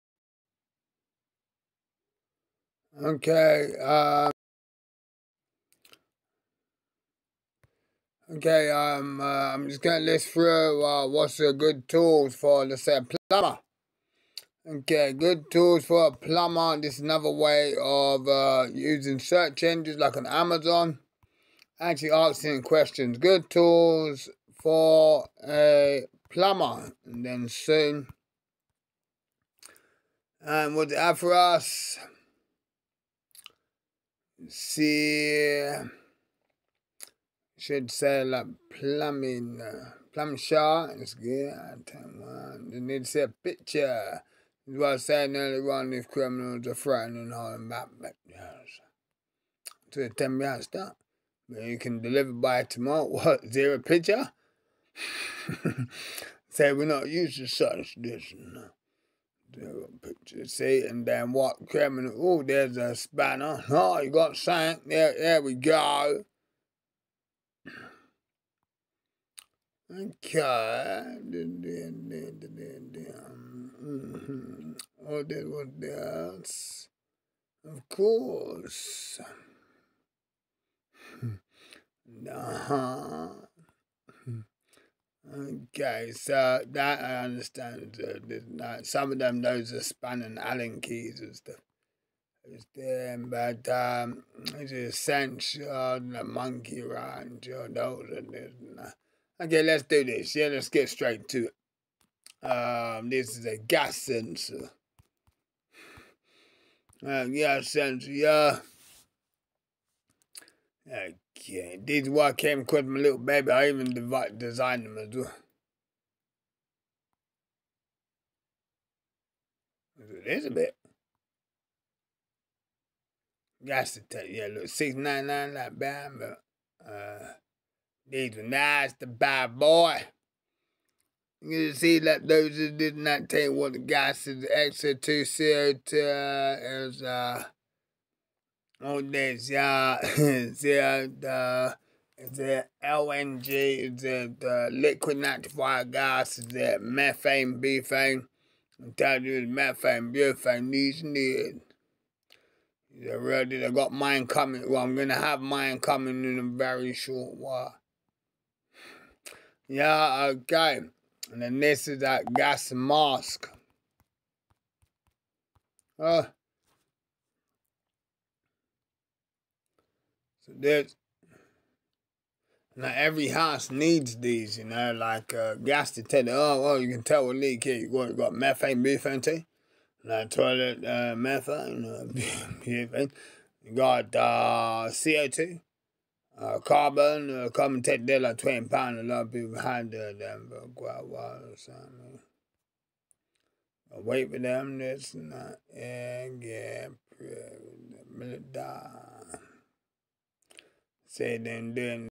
me f.o.o initials legally listing how people can search for things on shopping websites/ Amazon.com
me f.o.o initials legally demonstrating how to use shopping search engine websites indirectly as to literally ask " what is good for ...plumbers / age group / men or gender " to range of recommended or related as well as the normal mention some thing and it will take you to ...I think I first designed the system between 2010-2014ad. to pardon me for picture consealment that isn't Amazon.coms normal standards. . note tracking IME activities threw the system to I'm at canning town, London,e16 then it's the u.k / g.b then its the 9th august 2024ad how off from ad / BC can you be to 2000 years and mobile phones to local brands spoken about in normal language/s then watch out for such criminals as they all spies in the area to those illegally tapping in to fiber optics in the area talking about fiber optics the city amount needed and the recycle to provide that...to what the fat cats between London and new York talk and meow about...then let's finish with plagerism charges identifying spys to where did they get it from to how long have they had it to how long did it take to make to any copy right to interlectual property stolen / breeches related to it to especially if they ate believed to be a interlectual property theives... legal terms and conditions apply